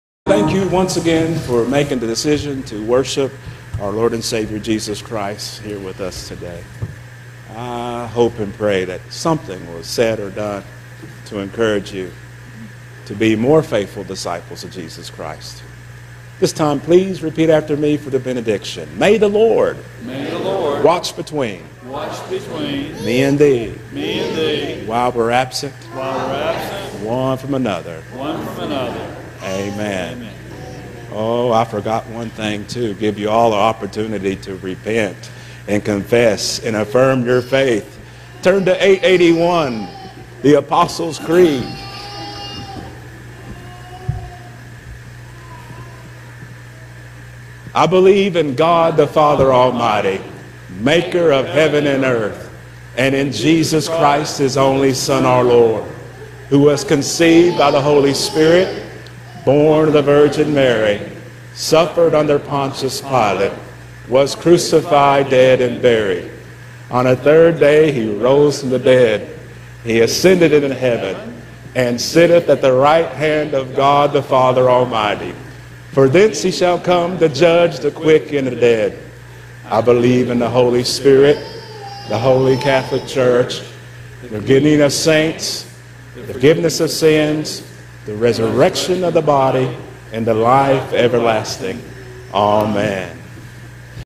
10272024 Benediction
Pastor Timothy leads the benediction and the congregational response closing out the worship service at Trinity United Methodist Church in Big Stone Gap, Virginia.